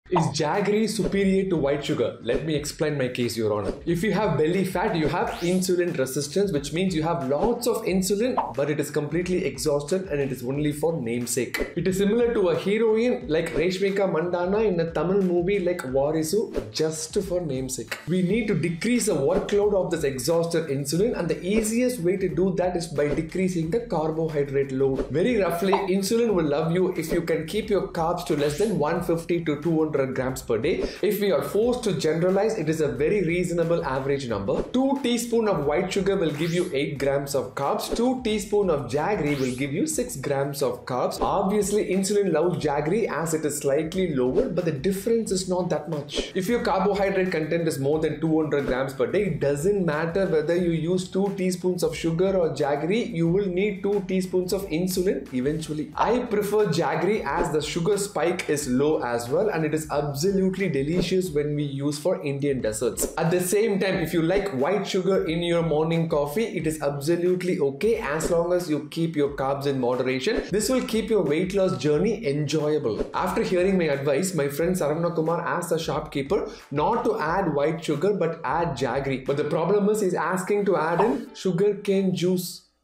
Is jaggery healthier? | Dr Pal
Jaggery is considered a superfood as it contains more vitamins and minerals and has low sucrose content when compared to white sugar. Jaggery is gaining popularity because it is more nutritious than sugar and offers various health benefits. Is it really a super food?